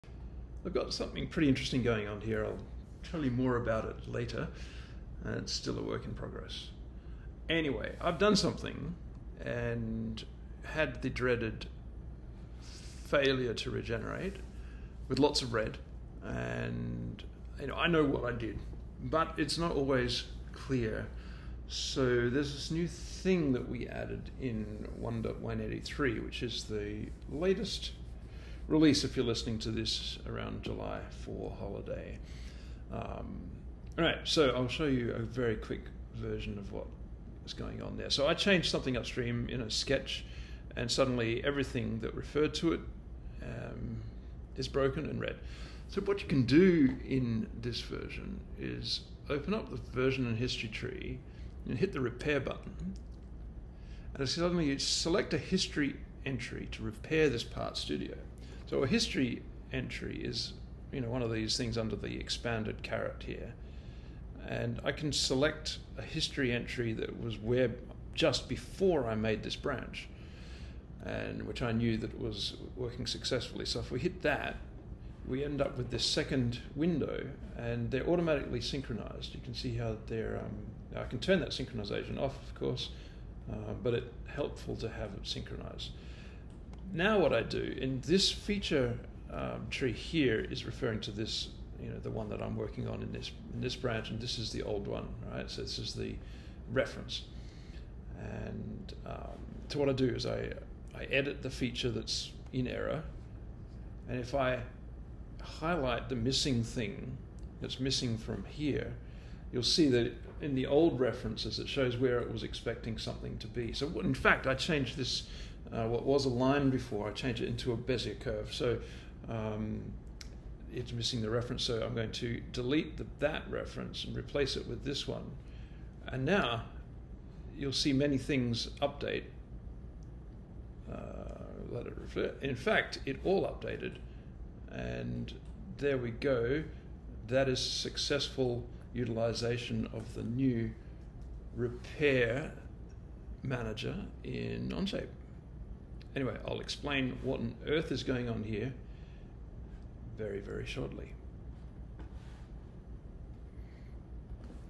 Onshape's new Repair manager - "What's broken can (now) be fixed"
A really quick one (for a change) - I was in the process of doing something else and came across a really simple but nice usage for the new Repair manager - something we introduced in Onshape's 1.183 release June 28, 2024.

The two-viewport visualization along with nice highlighting and synched view manipulation is very handy indeed. And since Onshape is... well... Onshape, it means that you can compare your broken part studio to any history entry that ever existed. From yesterday's session, from when you were working on a different computer, before that unexpected power outage... and so on. Unprecedented power - let's take a quick look!